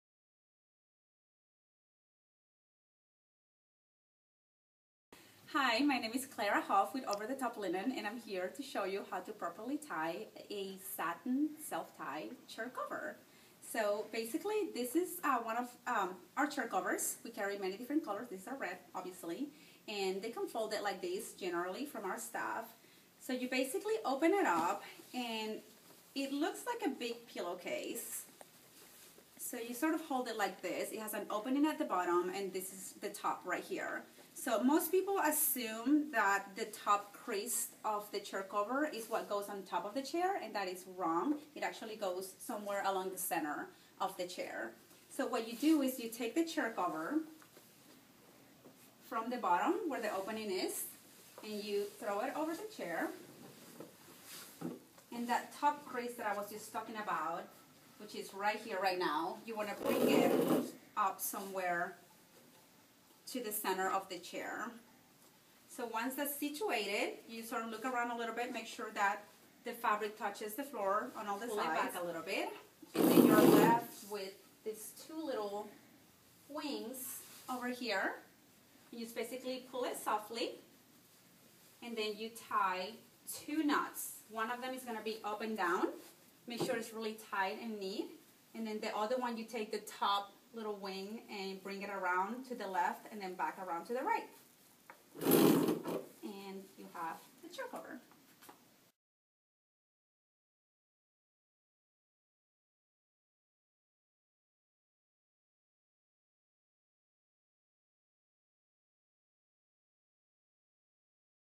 Using Self Tie Chair Covers
Clara with Over The Top Linen shows the En Vogue Events team some tricks on the best way to put a self tie chair cover on a banquet chair. Another installment in our FAQ's how-to videos.